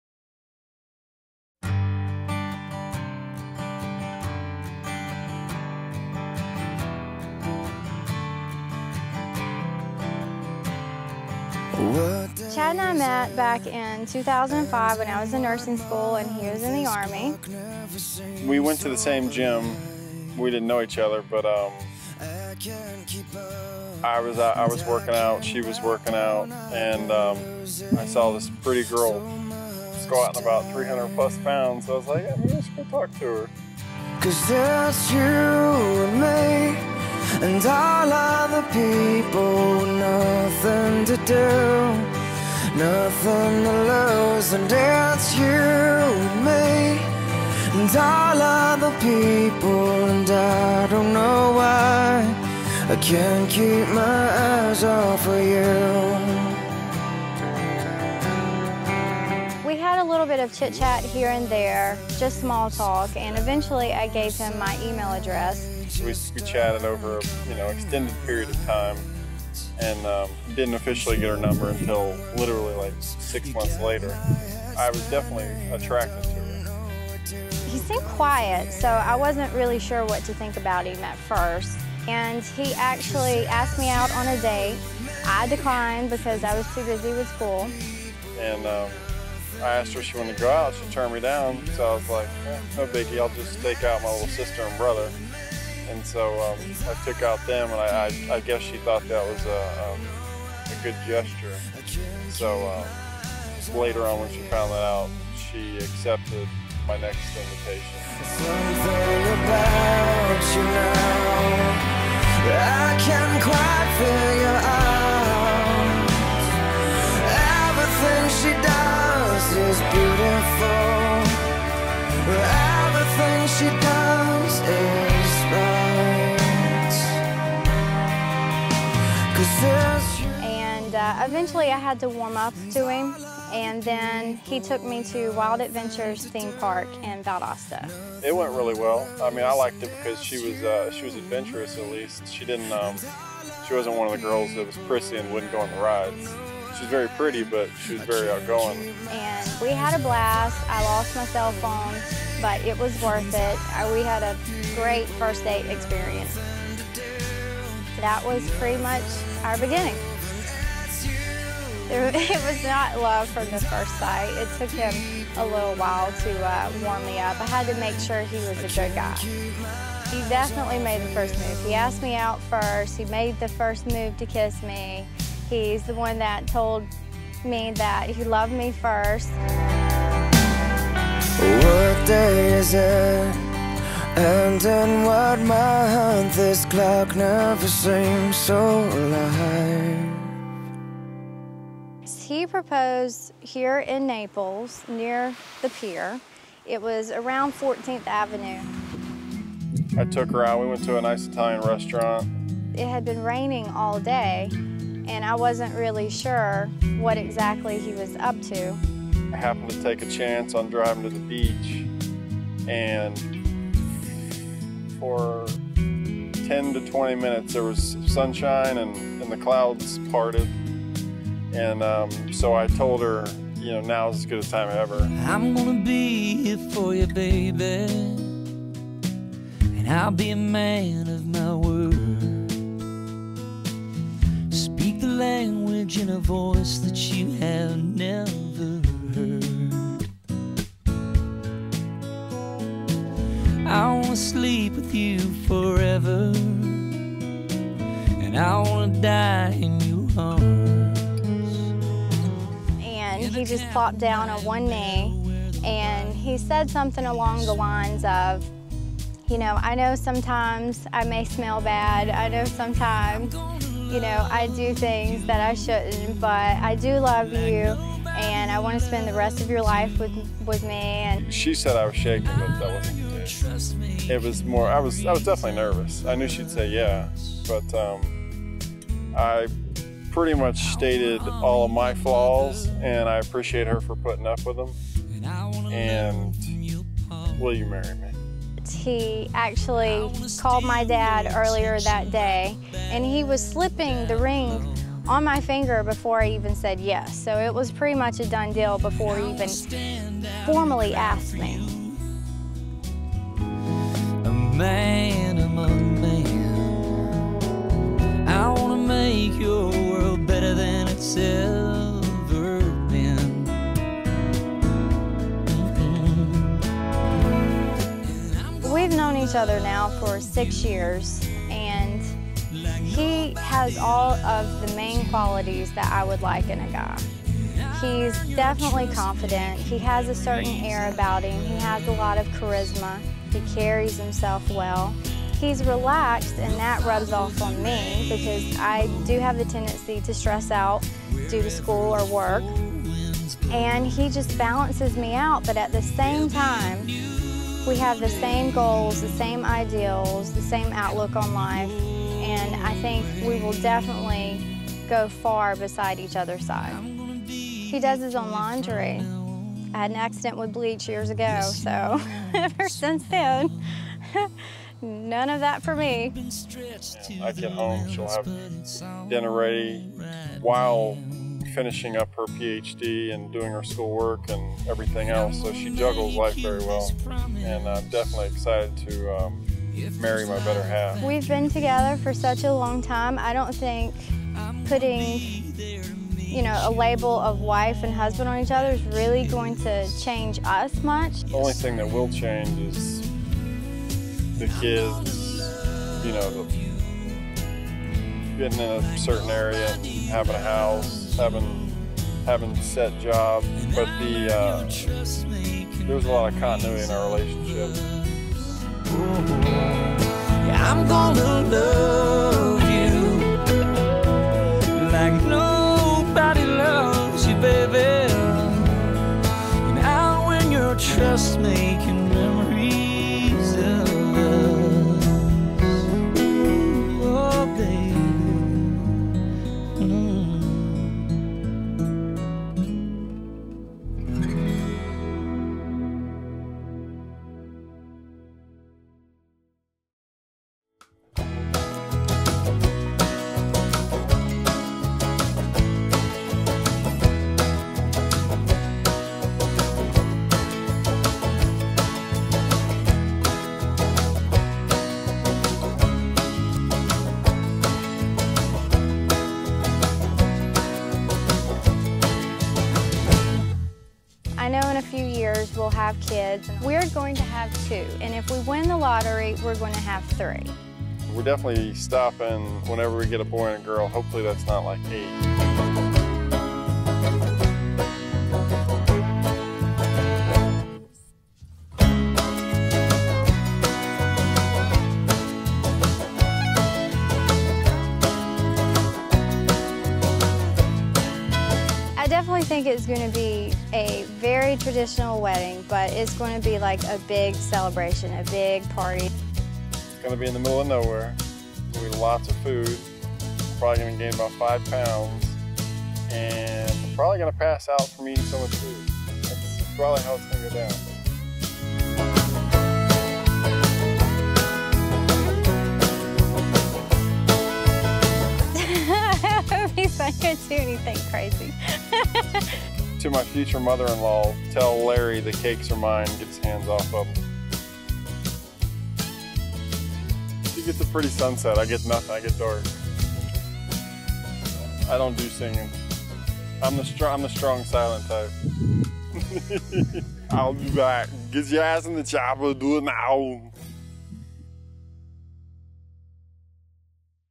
Best Engagement Video Ever! Brandi + Chad Love Story
Our friends Brandi and Chad met in a gym in Valdosta, GA back in 2005 where they were both living at the time. She was in nursing school and he was in the army. He was drawn to her nurturing compassionate nature and drive; she was drawn to his confident, laid back attitude and charisma. Even though they knew they were different enough to balance each other out, they also knew they were both on the same page with where they wanted to go in life. Fast forward 6 years, Brandi is working on becoming an Epidemiologist, and Chad is now a PGA golf professional here in Naples. Chad proposed shortly after Brandi moved down from Atlanta to Naples, exactly where our beach engagement video took place.